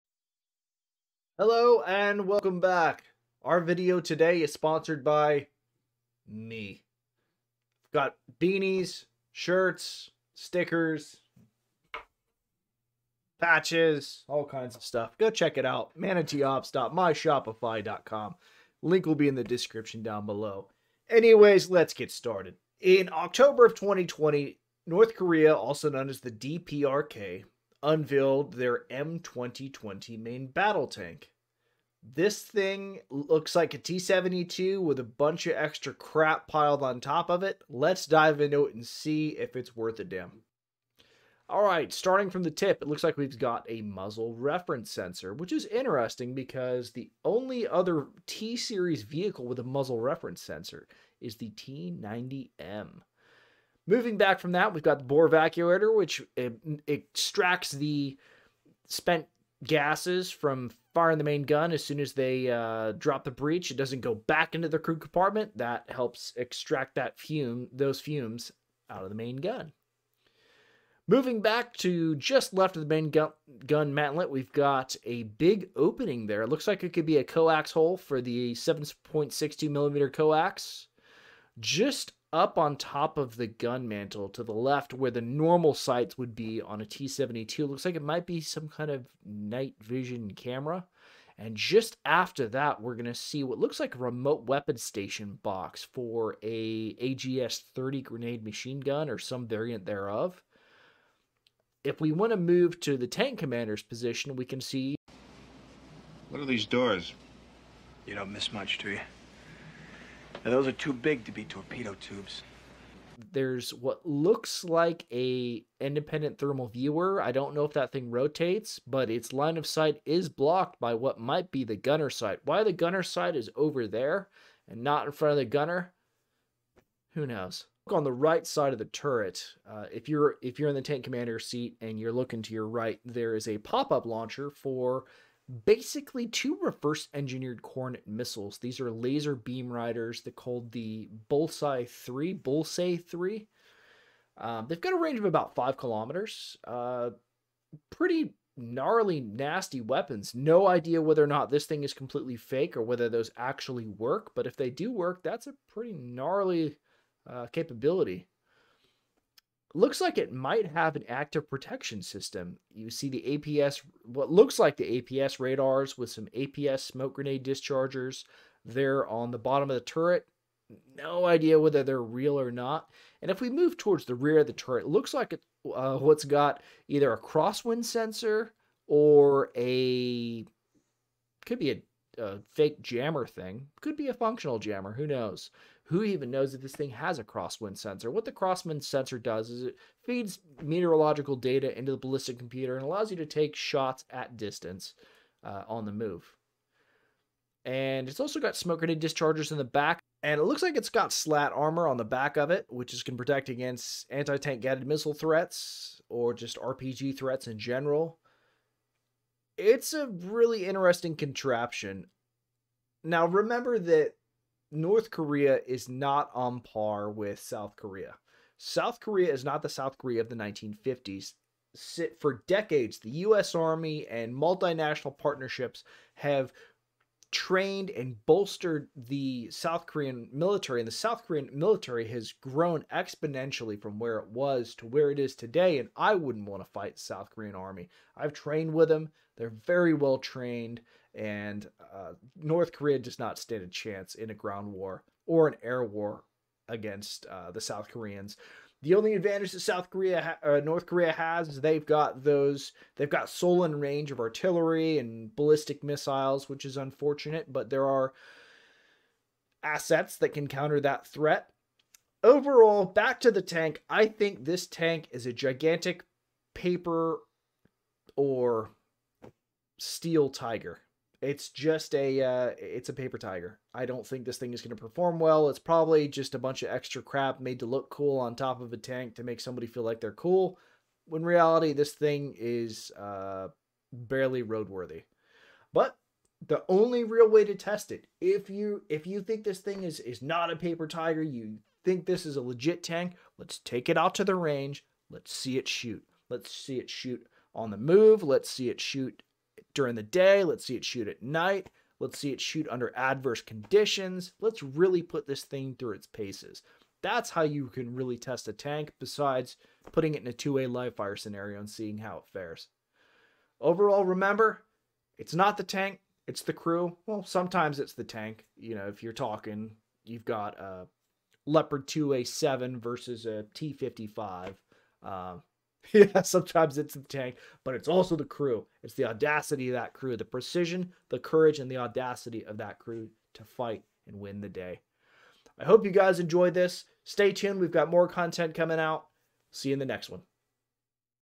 North Korean M2020 Breakdown
A Breakdown of the North Korean (DPRK) M2020 MBT.